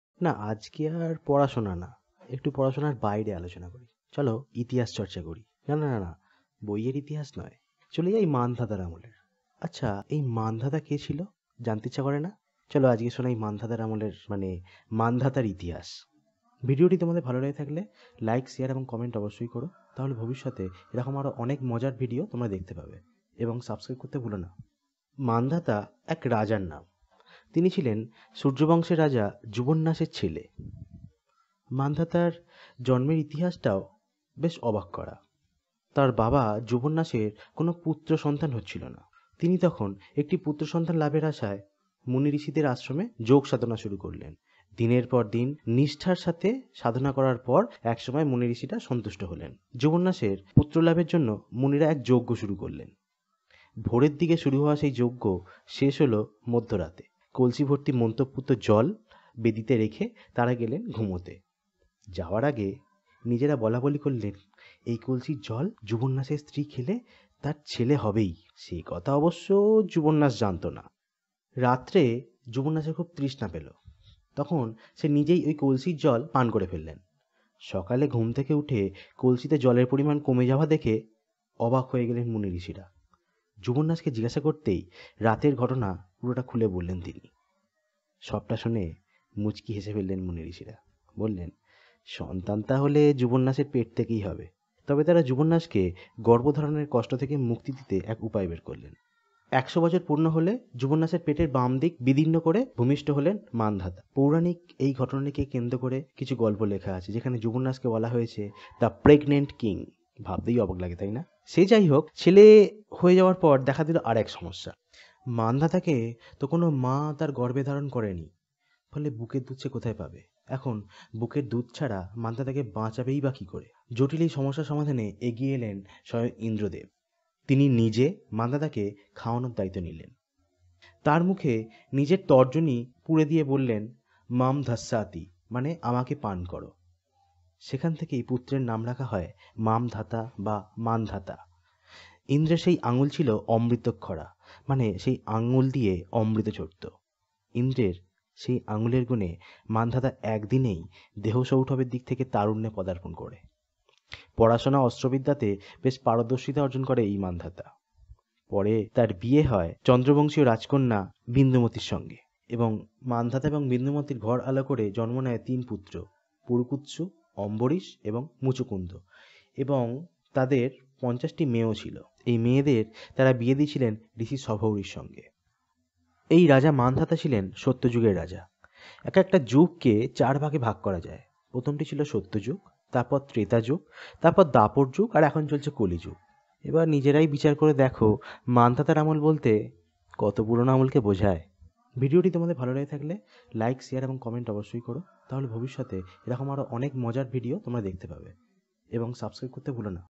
MANDHATA। কে ছিলেন এই মান্ধাতা?/ মান্ধাতার আমল
লোকে বলে মান্ধাতার আমল। কিন্তু কে এই মান্ধাতা? জানতে হলে চোখ রাখুন আমার এই ভিডিও টিতে। সবাইকে ভিডিও টি দেখার অনুরোধ রইল। ভালো লাগলে কমেন্ট করতে ভুলবেন না। আরো ভালো কিছু করার উৎসাহ পাব।